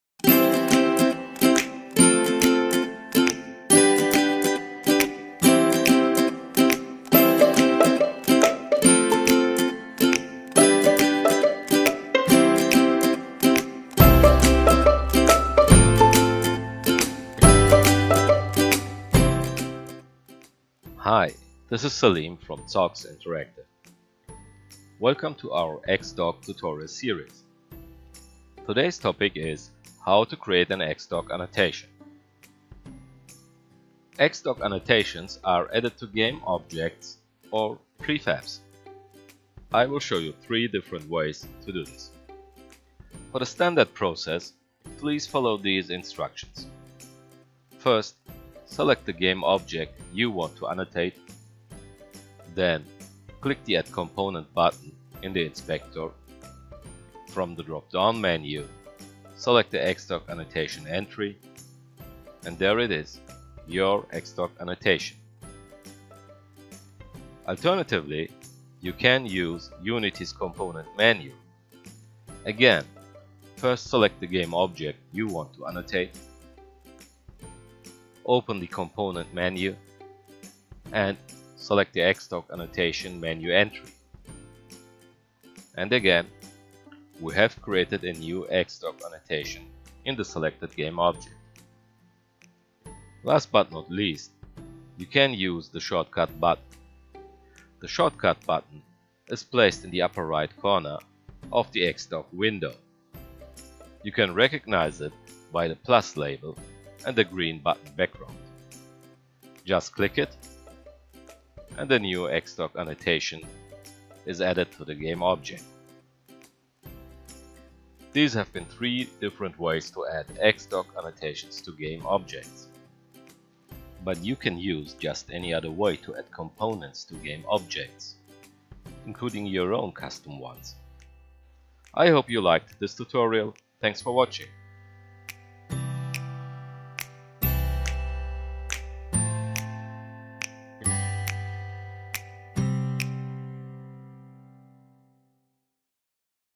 xDoc tutorial - How to create an xDoc annotation
xDoc, the best way to document your projects in Unity!
This tutorial shows how to create xDoc annotations.
xDoc brings embedded documentation into the Unity Editor!